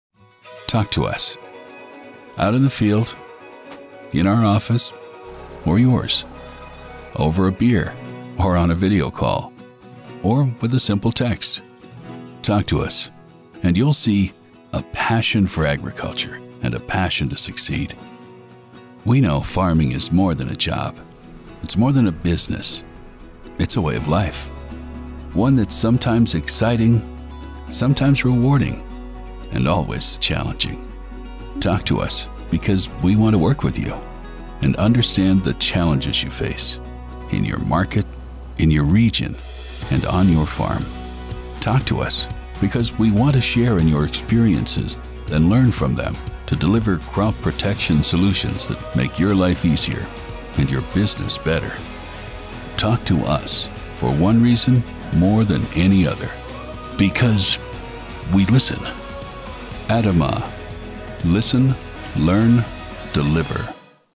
ADAMA: Talk To Us – Male Reading
Talk to us and you’ll notice it. A get-it-done attitude and passion for agriculture we all have. We keep things simple because we understand how hard this business is. But we also know that farming is more than just a business – it’s a way of life. Talk to us because your input helps us transform the largest group of actives in the world into the products you need.
Talk to us. We Listen. Learn. Deliver.
It’s as simple as that.